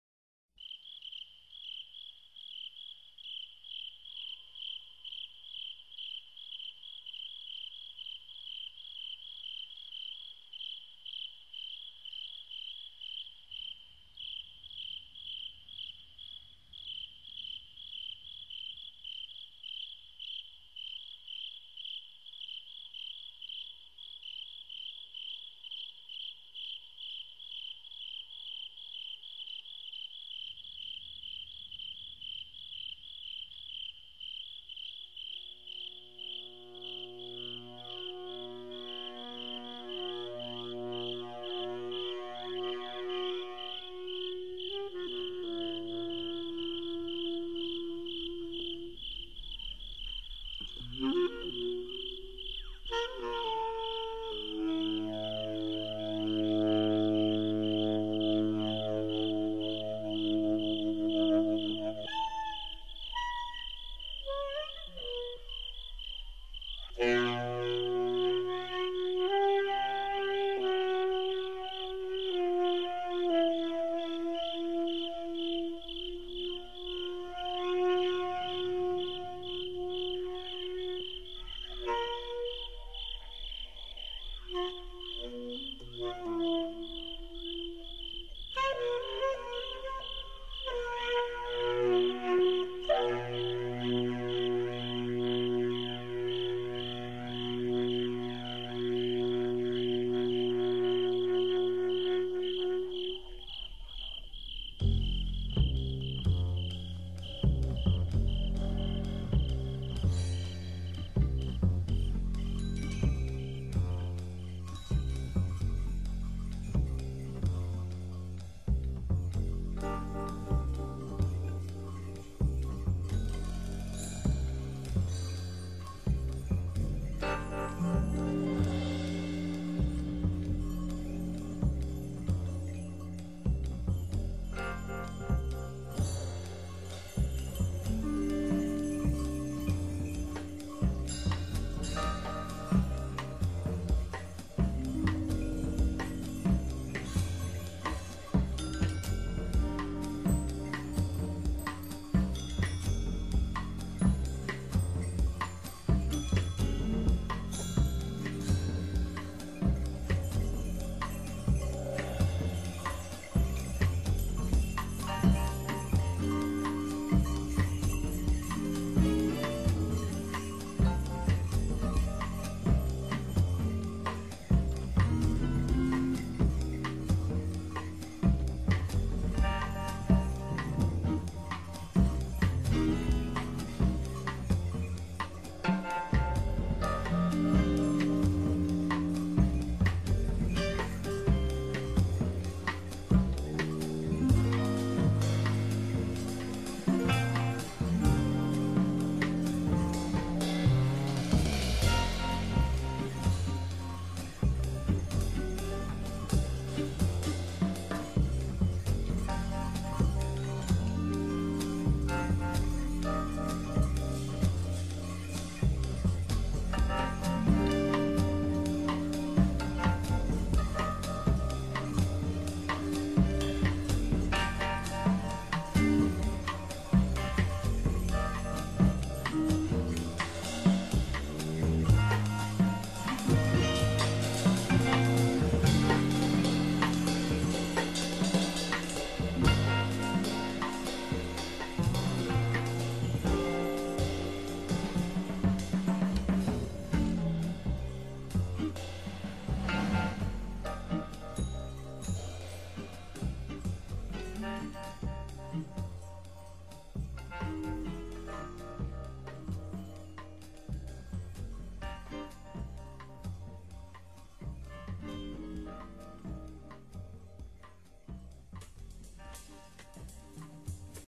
Egypt.wmv
This is a movie/picture gallery of our trip to Egypt - February 2010. It should be the first of a series of clips which are set to a single track of music and should become an audio visual album.